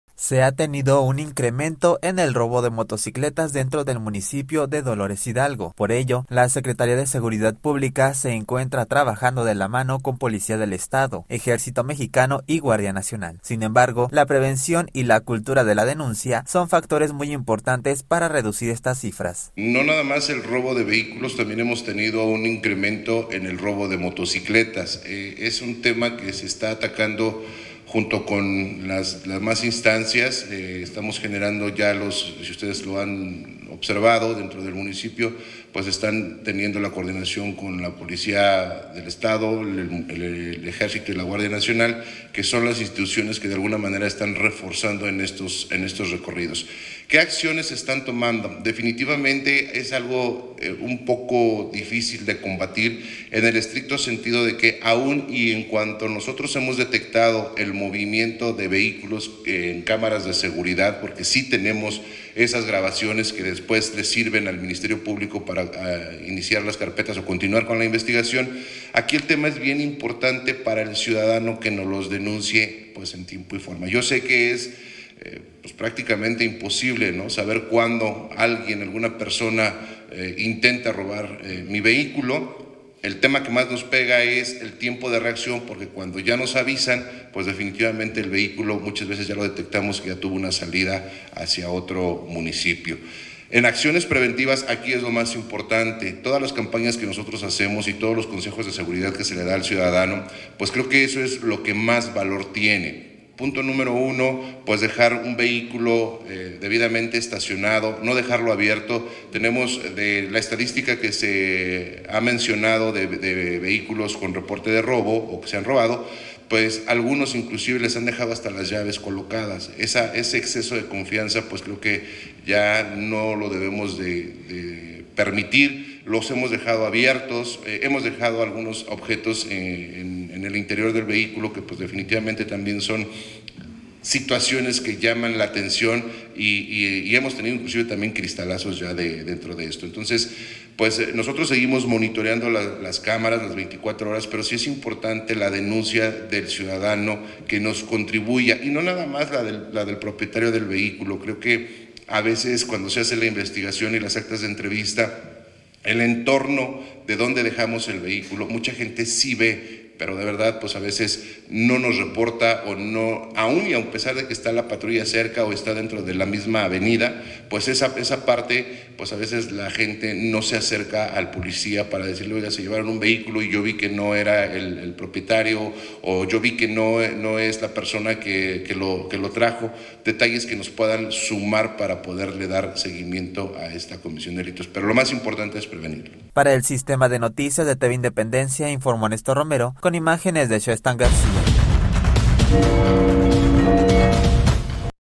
El robo de motocicletas ha tenido un aumento en Dolores Hidalgo
El robo de motocicletas ha tenido un aumento en Dolores Hidalgo, así lo declaró el Secretario de Seguridad Pública quién añadió que se mantendrán trabajando de la mano con Policía del Estado, Ejercicio Mexicano y Guardia Nacional para atacar está problemática.